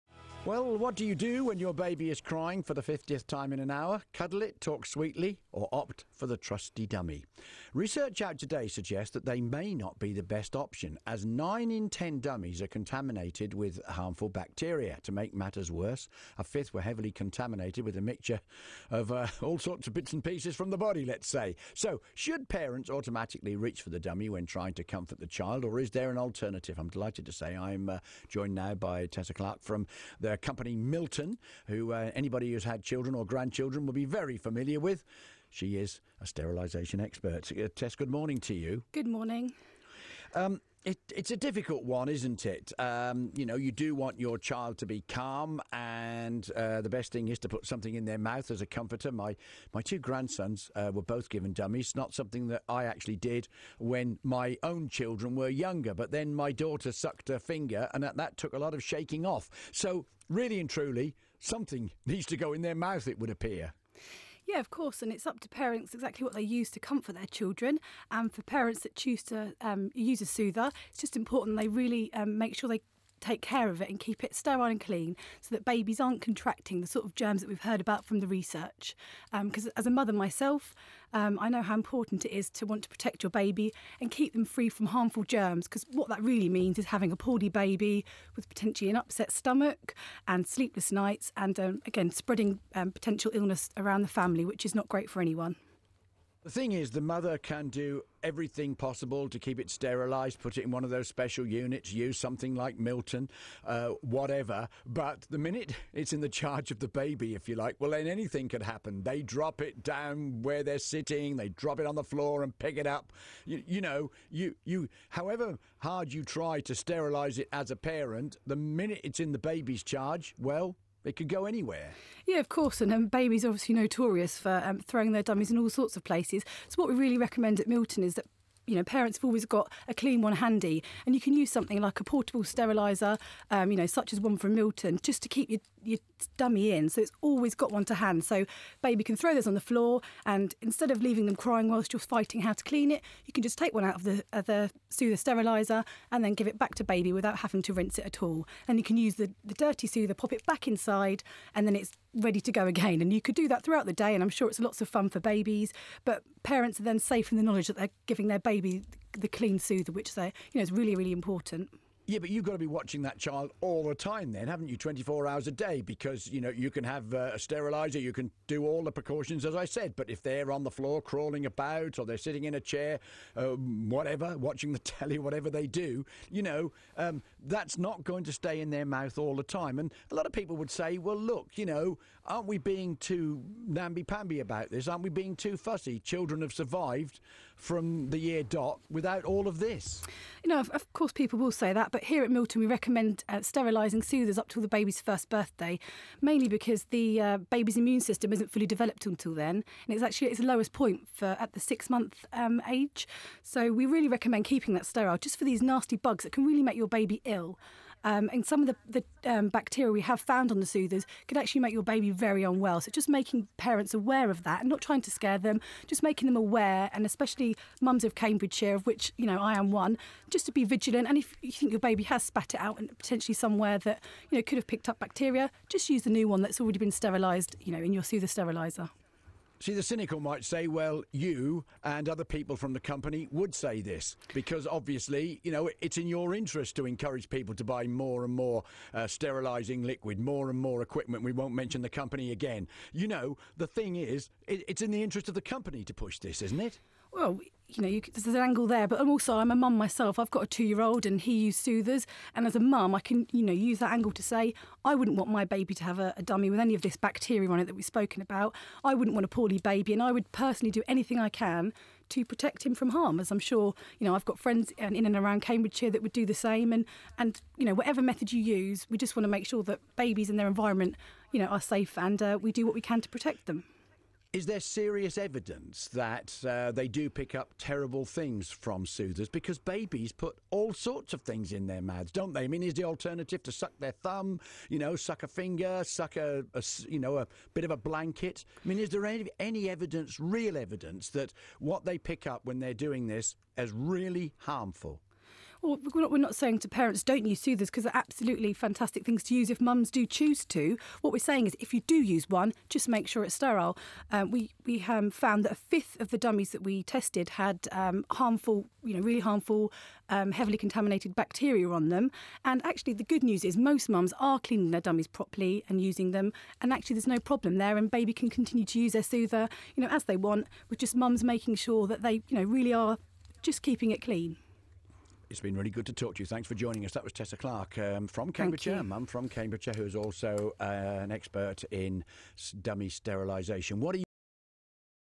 BBC Radio Cambridgeshire - Milton soother cleanliness research interview 20 Sept 2013
Milton's research into the cleanliness of dummies. 89% of soothers are contaminated, 1/5th are heavily contaminated. Radio Interview with Tess Clarke from Milton on 20 September 2013 for BBC Radio Cambridgeshire, discussing the research carried out by Dr Ronald Cutler from Queen Mary University of London for Milton.